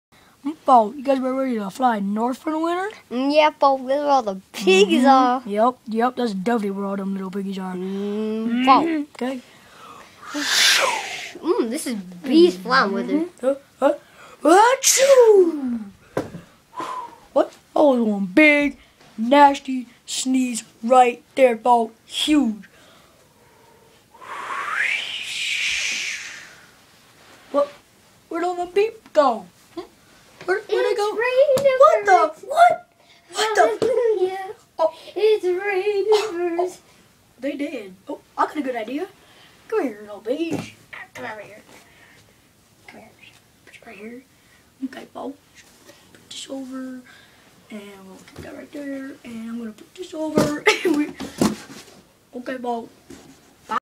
Angry Birds Go North
It's pig season and the pigs don't want miss one second until Black Quail gets allergies...